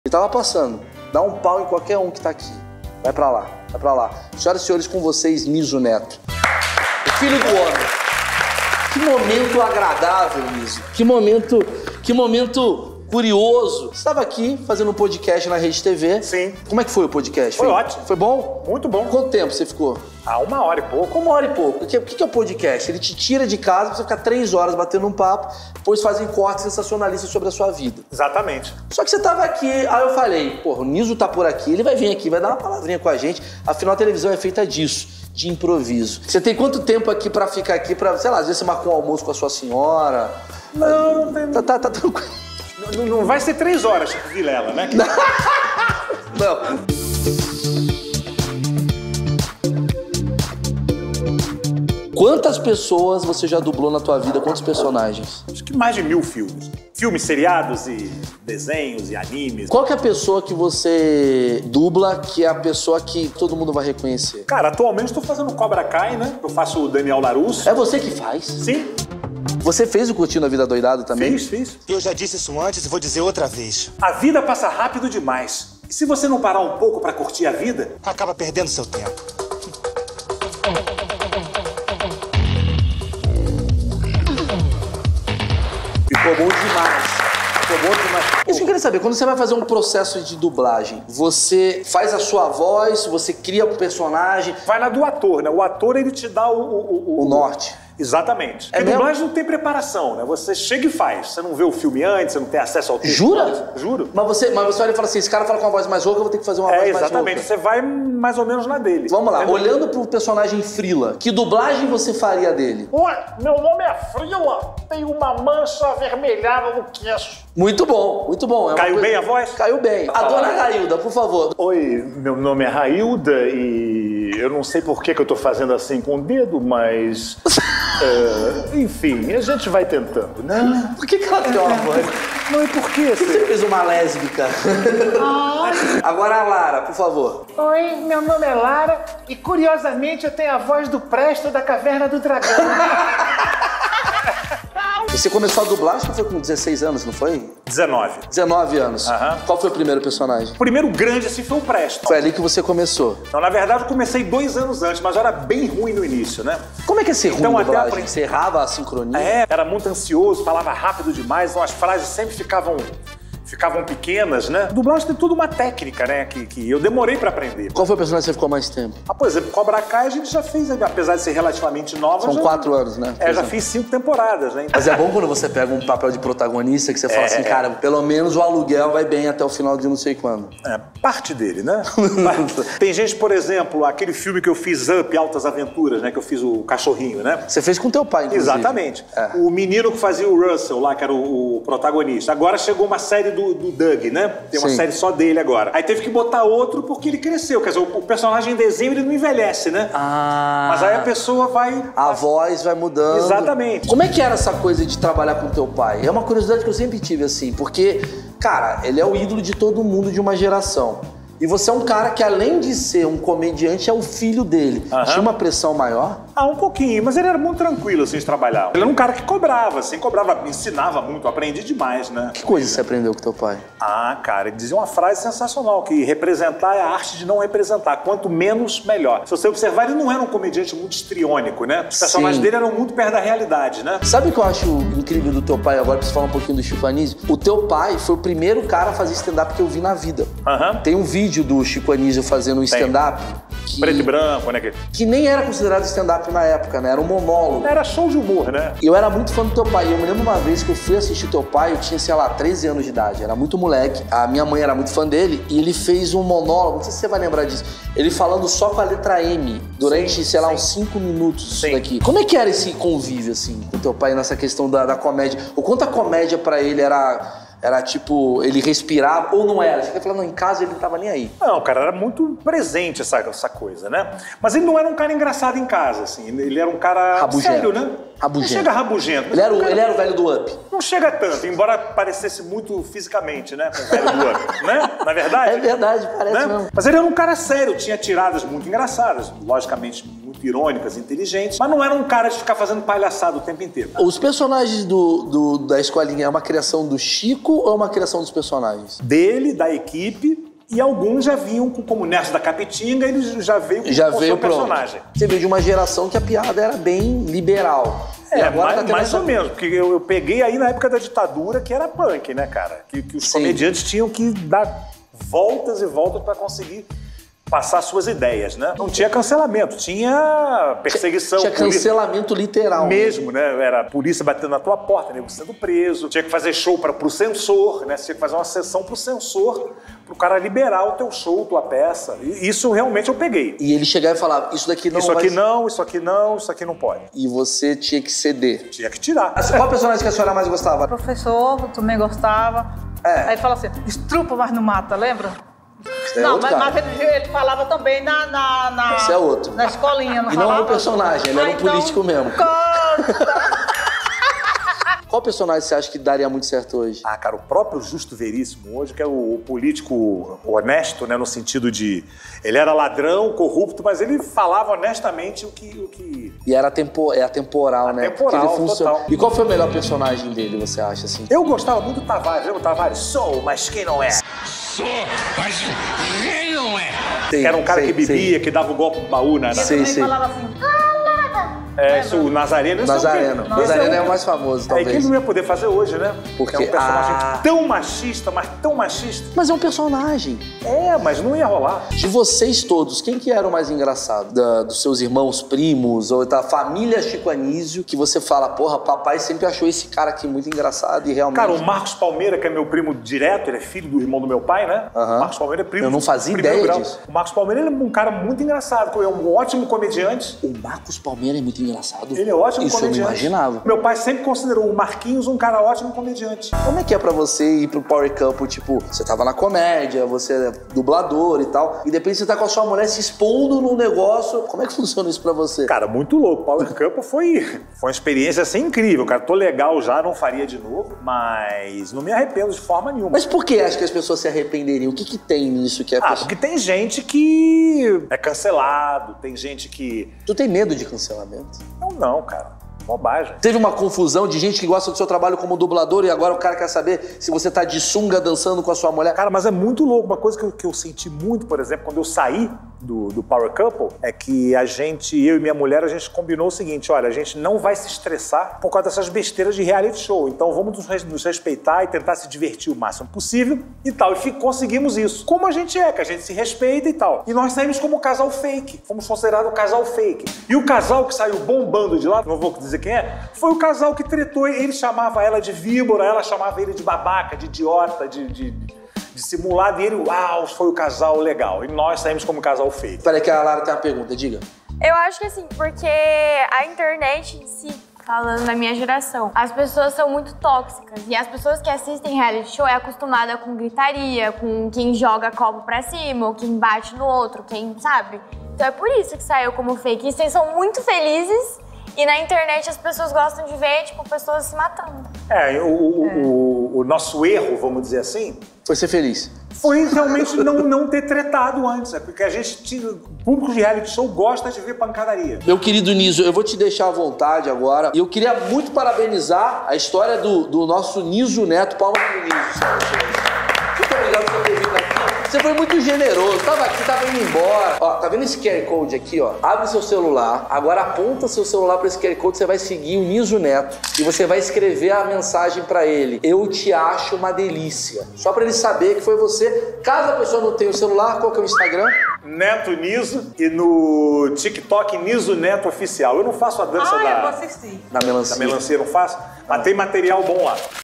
Entrevista com NIZO NETO - FOI MAU
O dublador e humorista, Nizo Neto, contando as maiores dificuldades de um humorista na ditadura e relembrando do seu pai, Chico Anysio.

O programa que MELHORA a sua semana, pode melhorar TODOS OS SEUS DIAS!